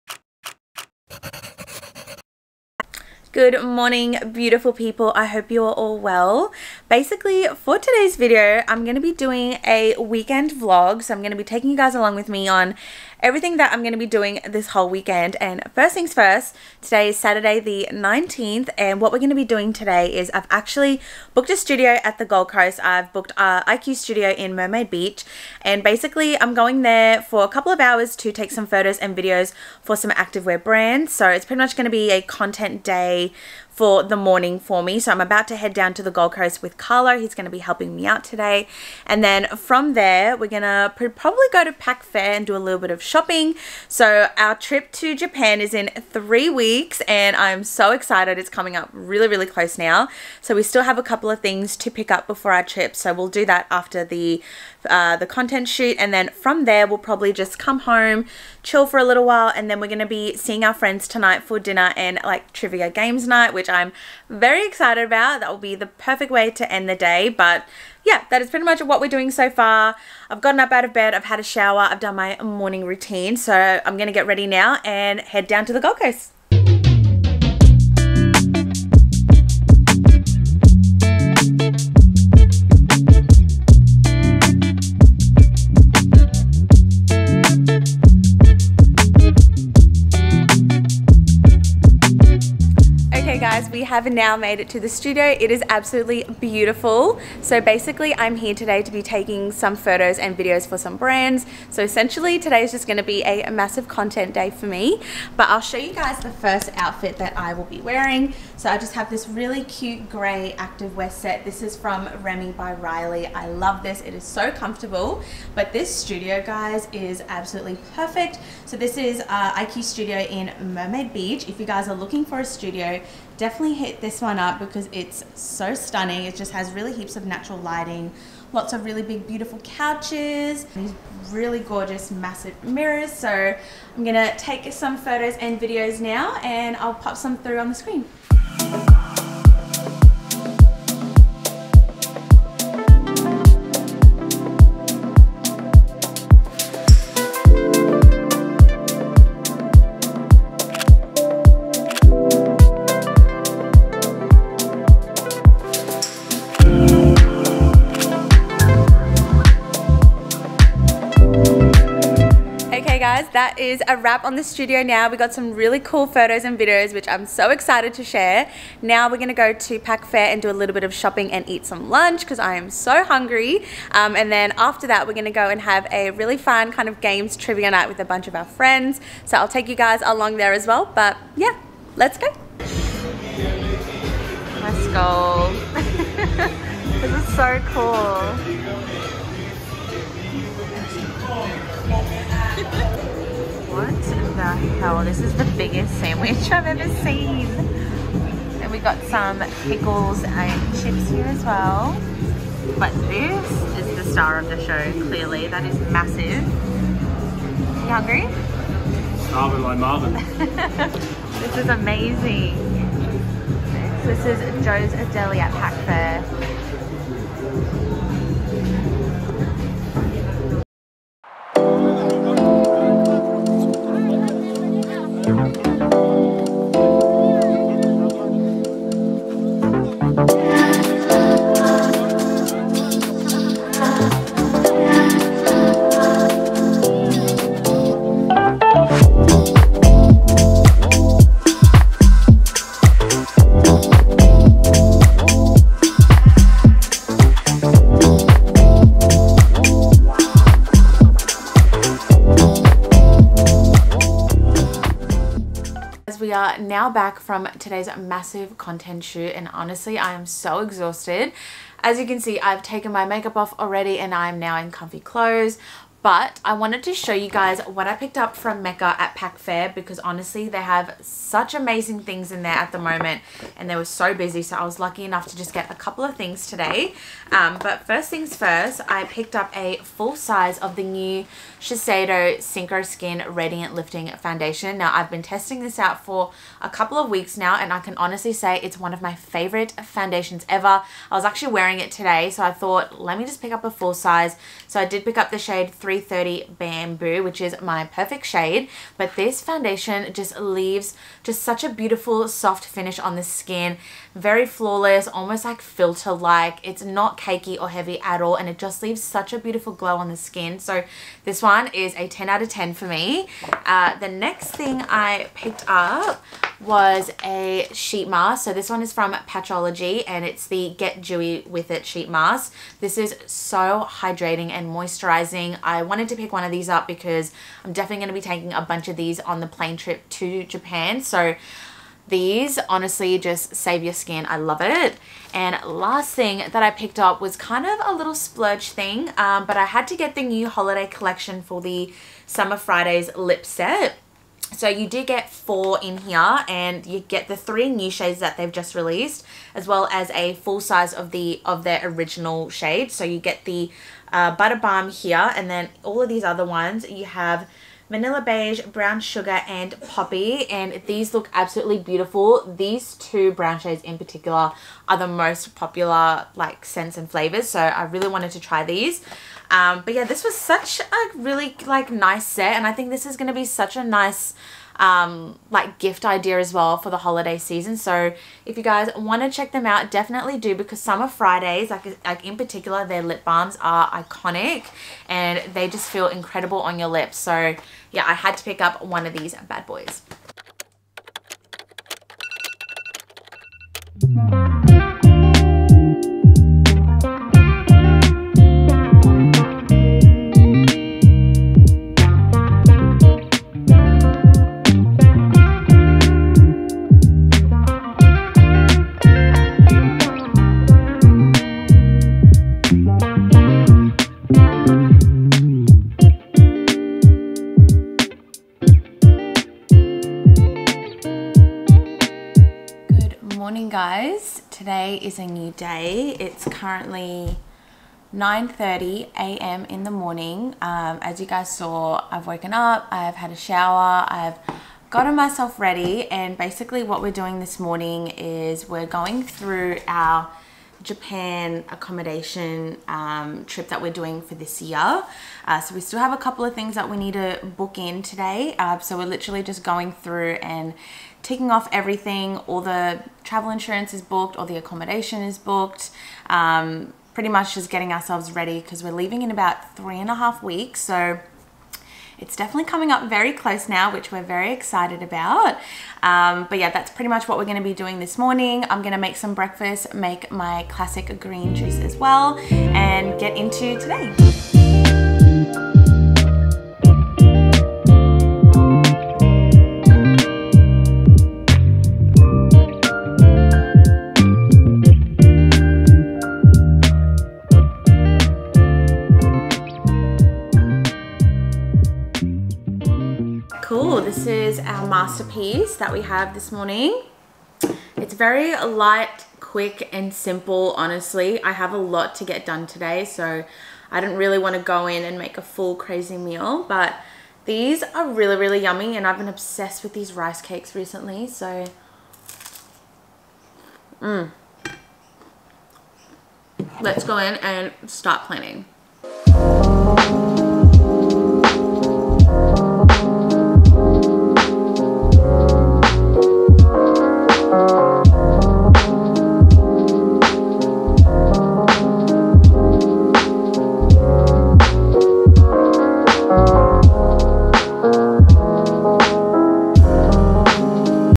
Busy & Productive weekend vlog | GRWM, Mecca haul, content day and planning for JAPAN
Hey angels 💞 in this weekend vlog, I take you guys along with me for a studio content day, shopping, and planning for JAPAN !!